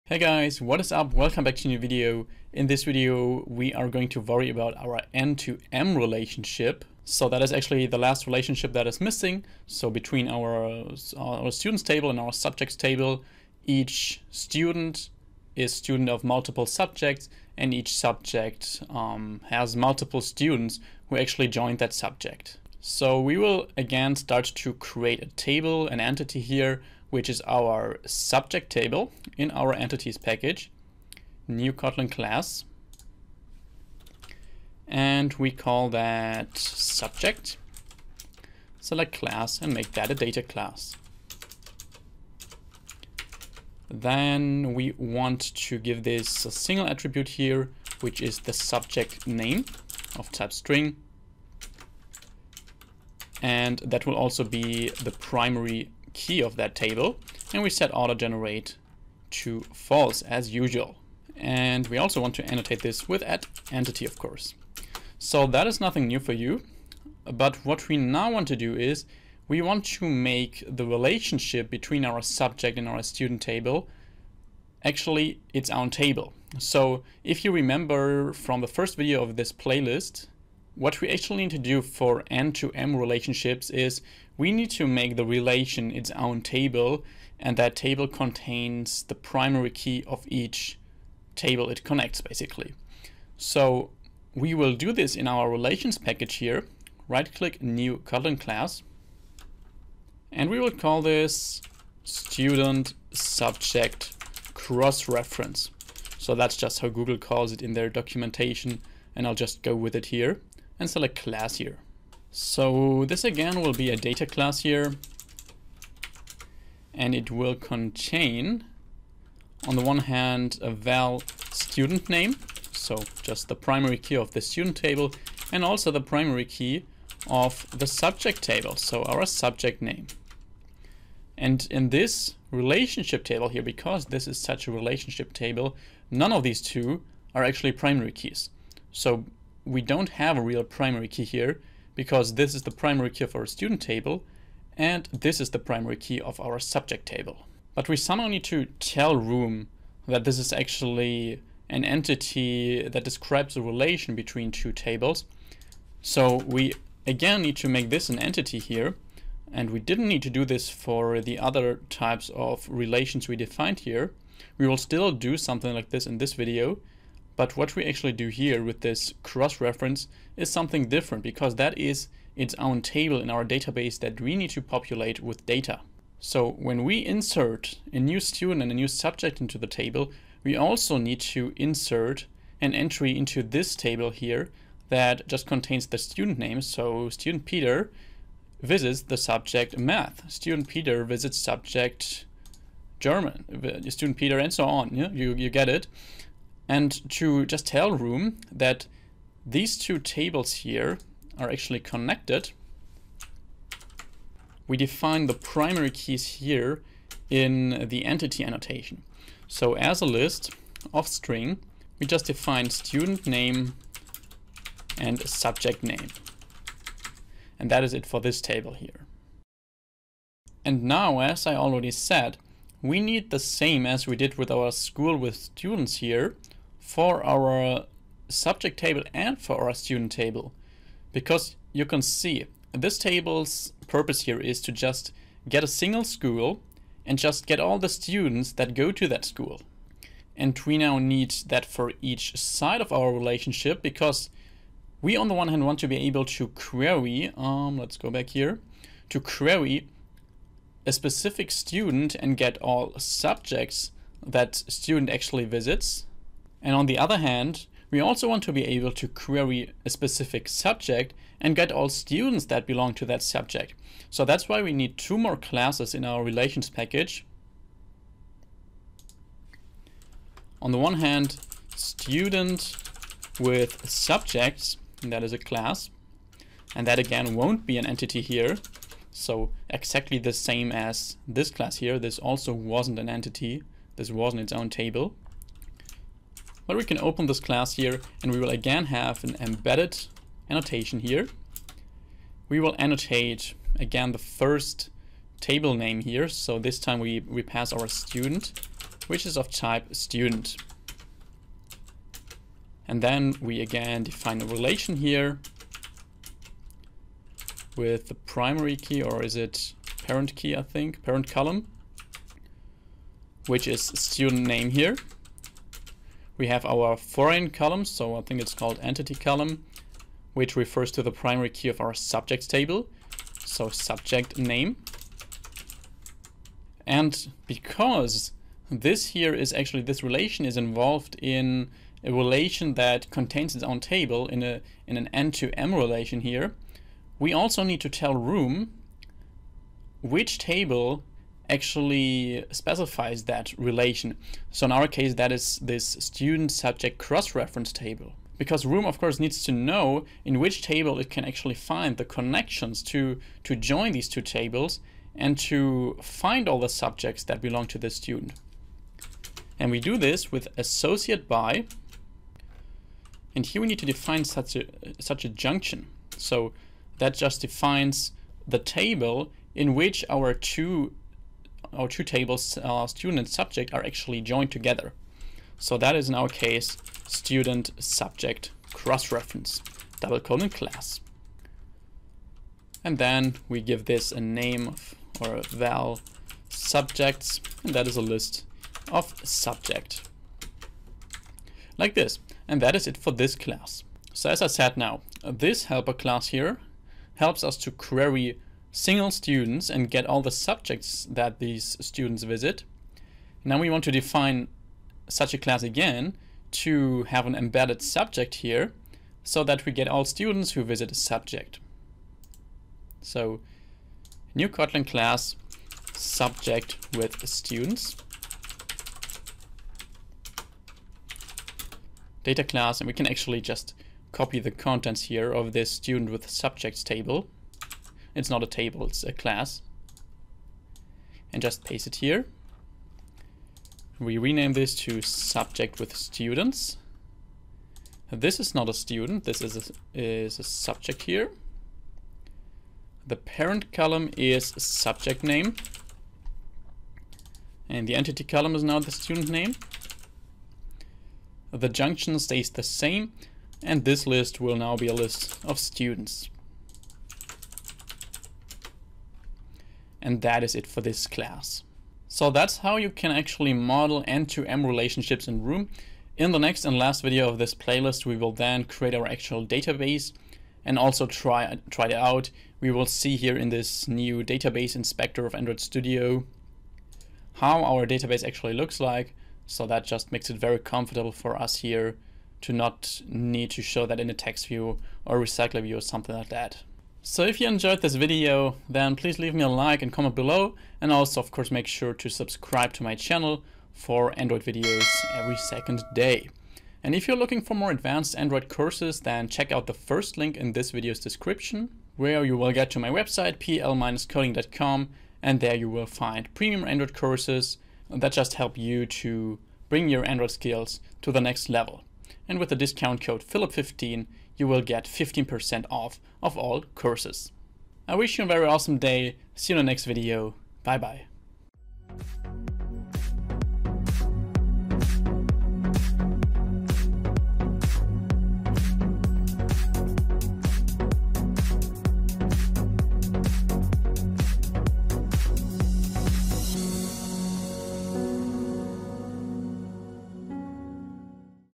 N-to-M Relationships - Android Room Database With Multiple Tables
In this video we will create two versions of our repository to also efficiently use it in our app.

⭐ Get certificates for your future job